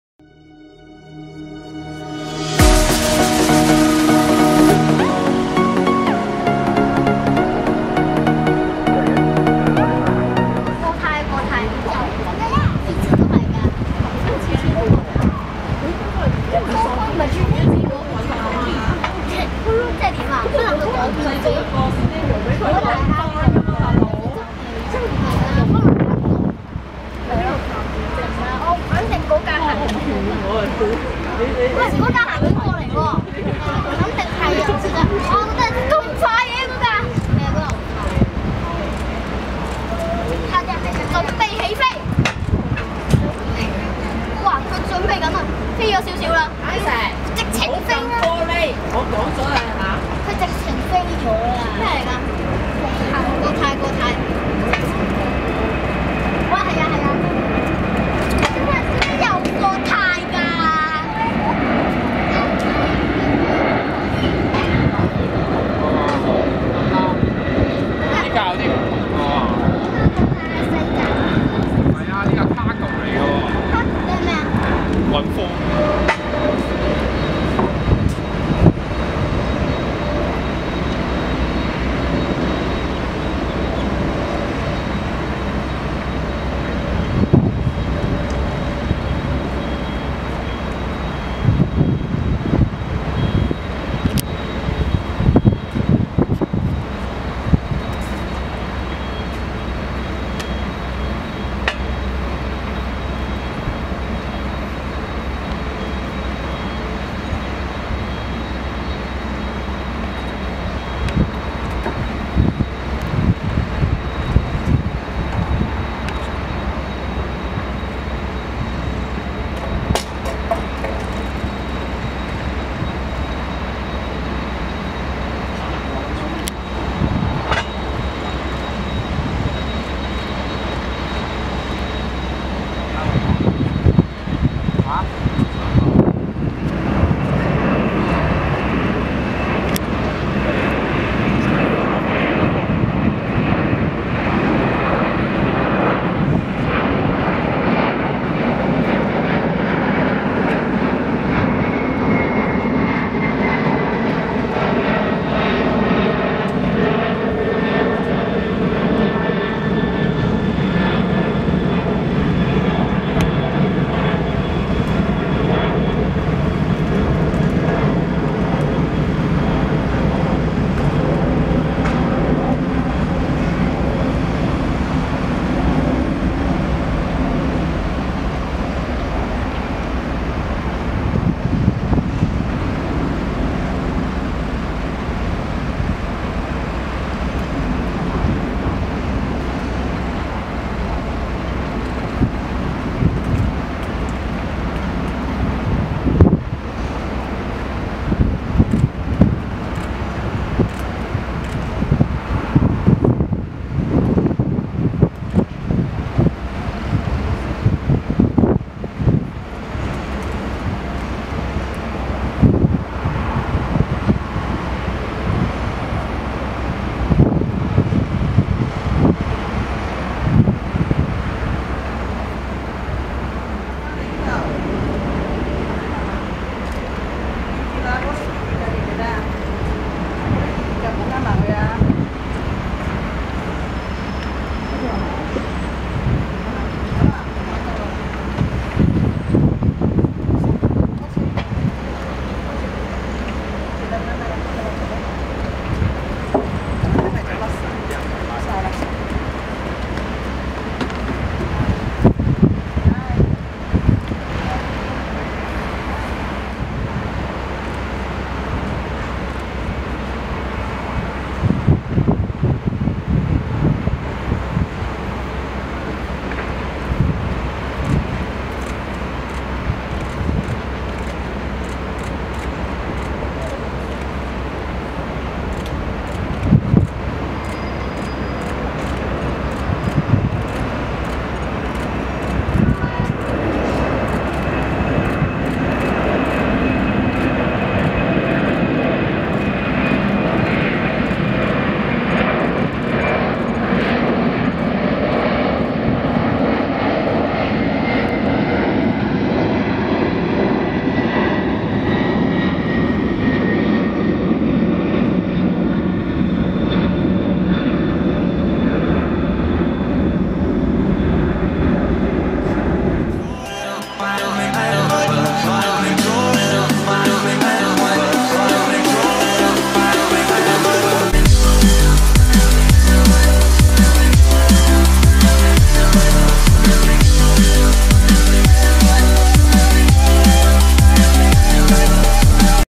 747 Plane Spotting at Hong Kong Airport
This video is of many different Boeing 747's arriving and departing Hong Kong International airport.

This video was taken on top of the Skydeck which is located on top of terminal two at Hong Kong airport. The video was taken using a Canon 5D mark 3 using a 70-200mm lens as well as a 2 times converter.

If you would like to see photos from this spotting trip, you are able to see it on my Instagram which is linked below: